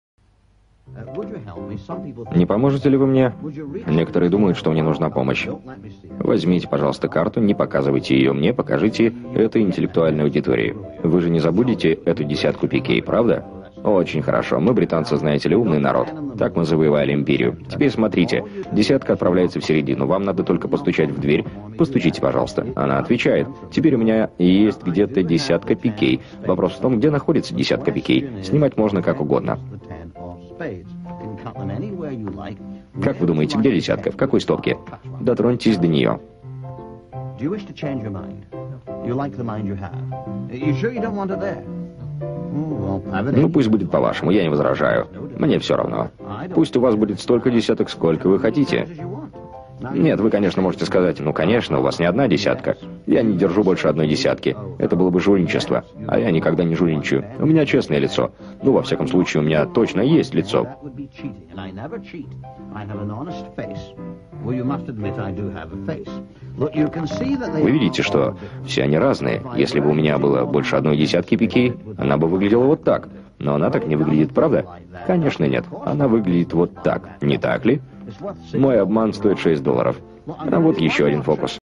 The Svengali Deck in Russian!
This is Mark Lewis performing the Svengali Deck and it is translated into Russian. Oddly enough Mark is of Russian ancestry!